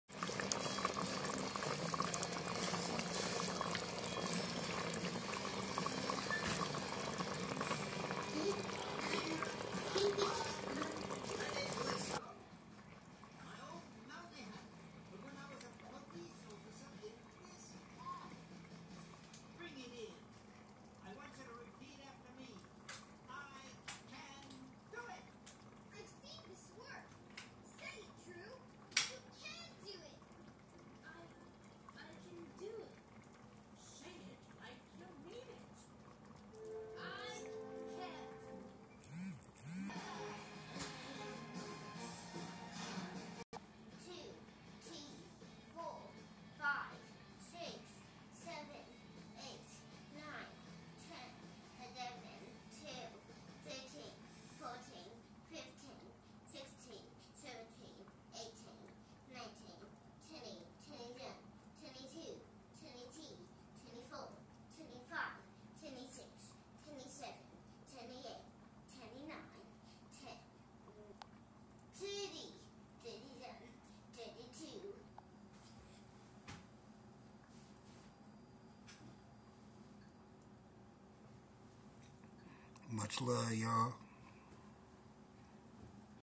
Fall Off The Bone OXTAILS Easy Quick Meals For Single Parents
Signature Braised Oxtails
The Most Flavorful Fried Plantain
Citrus Pollo Asado Southern Dirty Rice
Savory Candied Yams
Buttered Cornbread
HAVE A GREAT FRIDAY Y'ALL
NEW VIDEO LOADING NOW.
Much Love Y'all
'24 Take Full Advantage💯 Cookbook Coming.